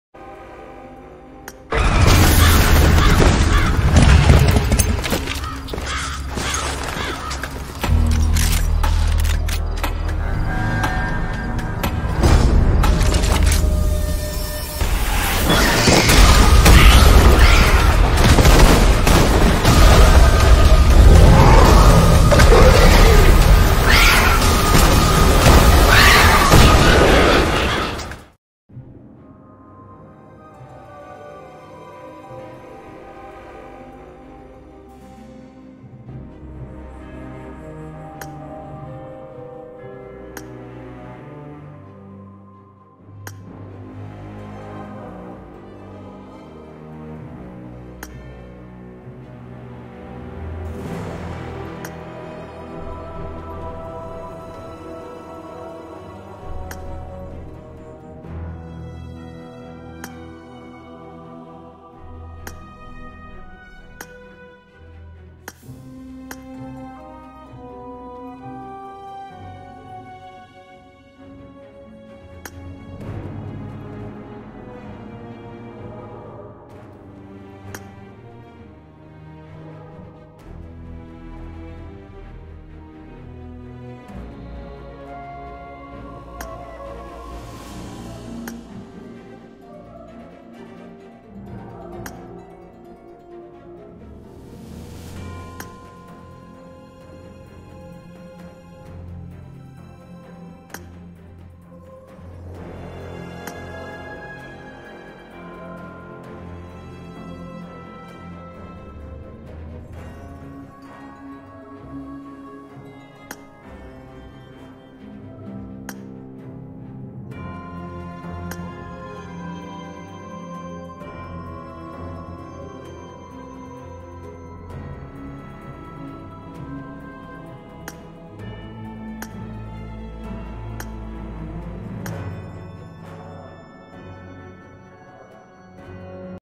Fright Night Premium Crate ~ time to scream!!
Halloween themed crate, the fright night suit and new helmet etc, bike skin , backpack and kar98k skin